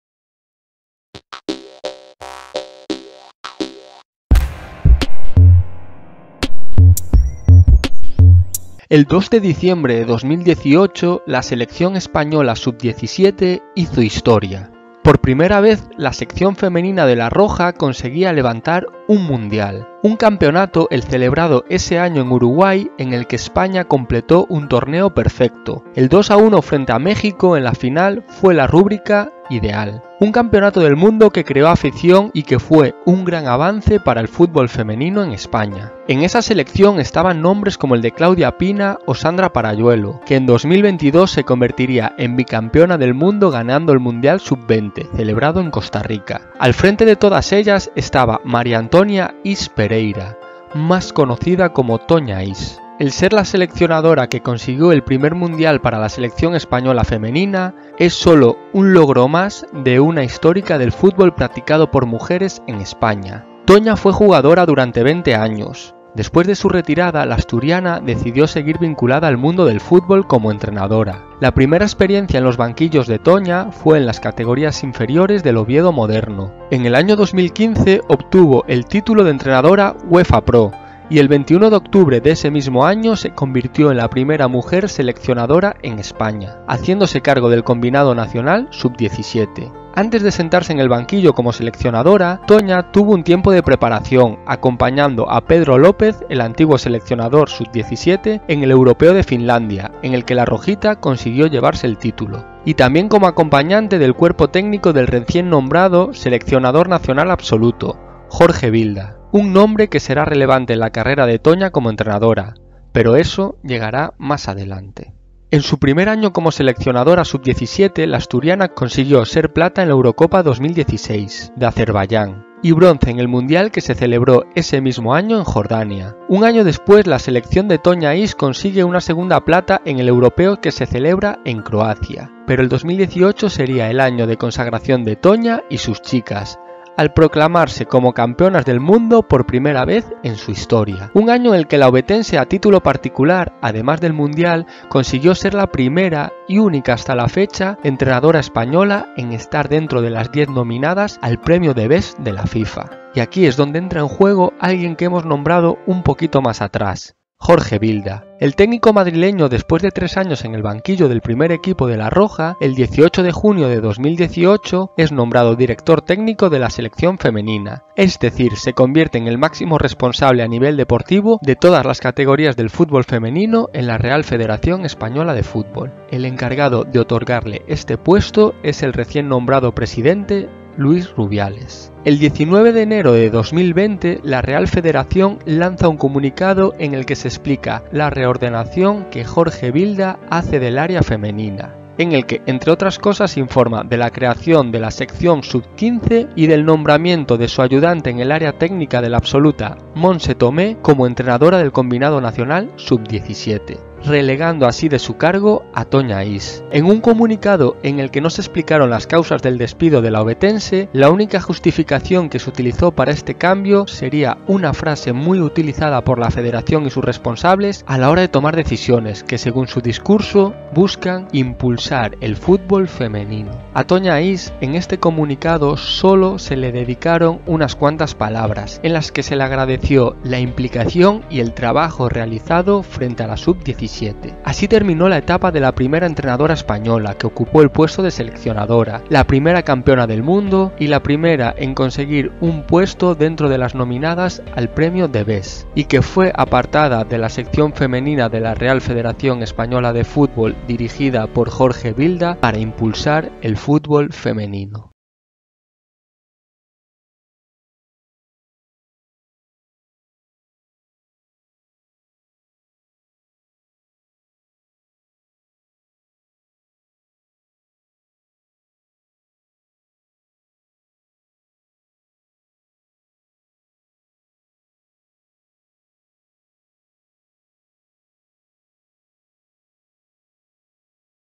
JORGE VILDA su IMPULSO al FUTFEM. El DESPIDO de la PRIMERA SELECCIONADORA CAMPEONA del MUNDO.
Episodio dos, serie "Las polémicas de Vilda"